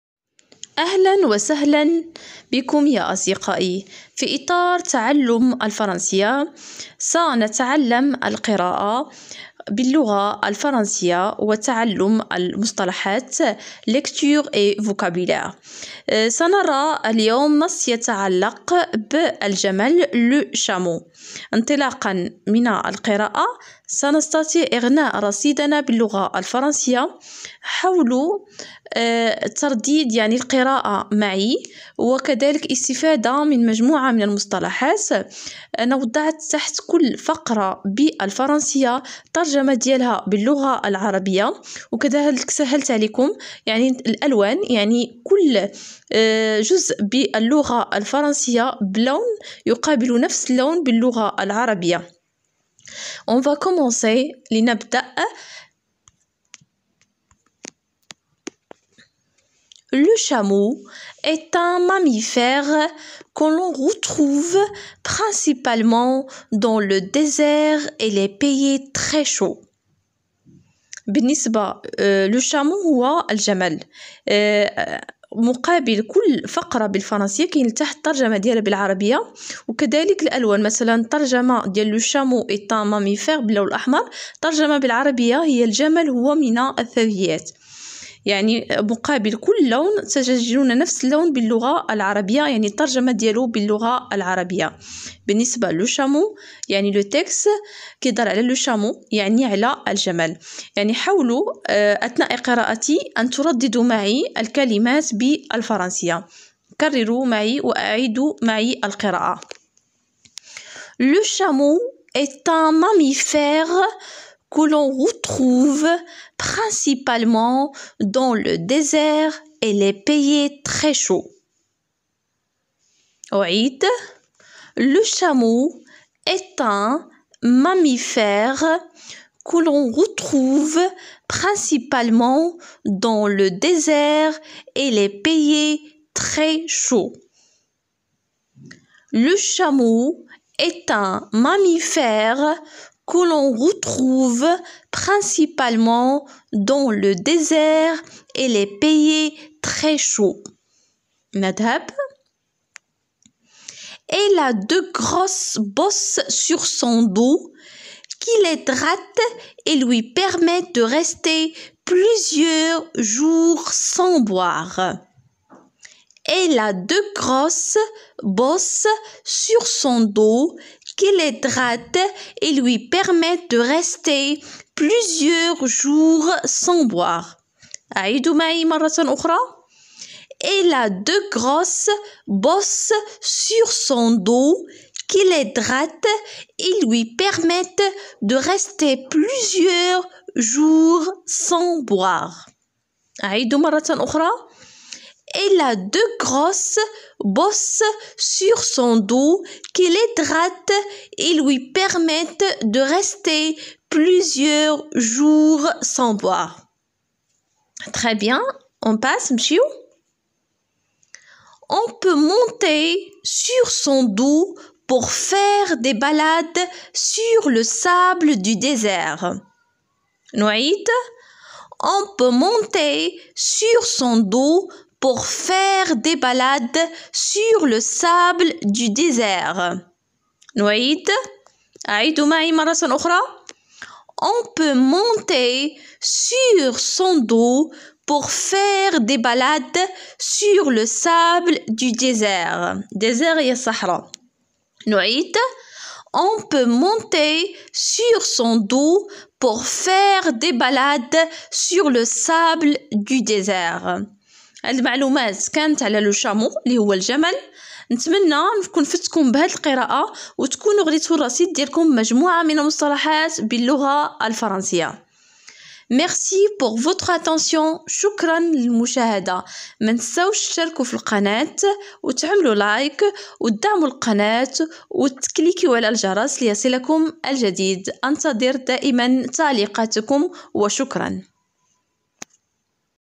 apprendre la langue française lecture et vocabulaire le chameau تعلم الفرنسية القراءة والمصطلحات جمل
تعلم الفرنسية القراءة واغناء الرصيد apprendre la langue française lecture et vocabulaire le chameau